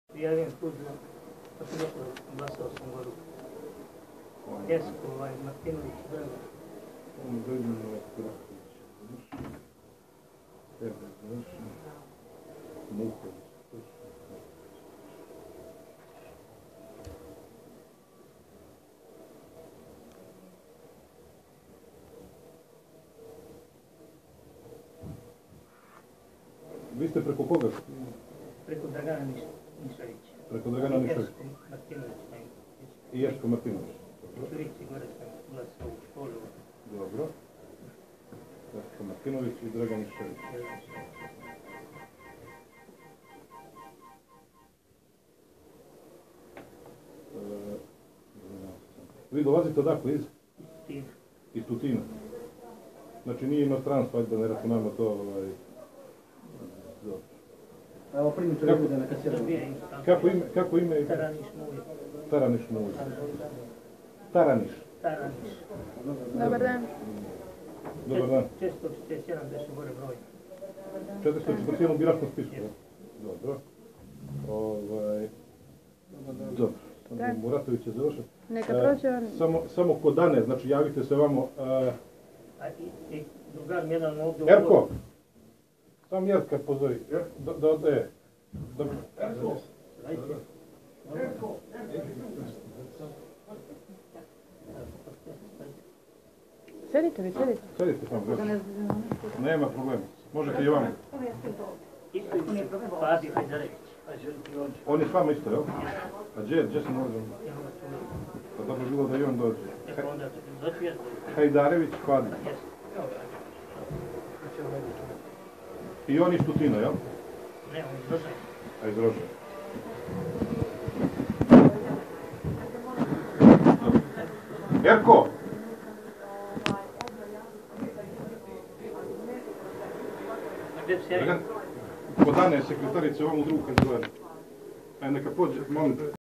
250 eura glas za DPS Taranis Mujo iz Tutina (Srbija), 16.10.2016. Preko Eska Martinovica.
Dakle, u cemu je ovdje fora: Kancelarija opozicionog DF-a je tik uz  kancelariju Milovog DPS-a. I dovitljivi aktivisti DF-a, koji znaju da kupljeni DPS-ovi biraci iz dijaspore dolaze po novac u kancelariju DPS-a, uvode ih u kancelariju DF-a, gdje ih docekuju Milenko Jovanovic i Bogic Beli Mitrovic koji simuliraju celnike lokalnog DPS-a. I ovi im hladno priznaju koliko im je DPS para obecao, koliko  su vec uzeli, odakle su dosli da bi glasali za DPS i slicno. Nakon sto im kopiraju pasose i uzmu sve podatke, onda ih salju u stvarnu kancelariju DPS-a da preuzmu novac, gdje novac i preuzimaju. Dakle, nevjerovatno raskrinkavanje masovne kradje i kupovine glasova.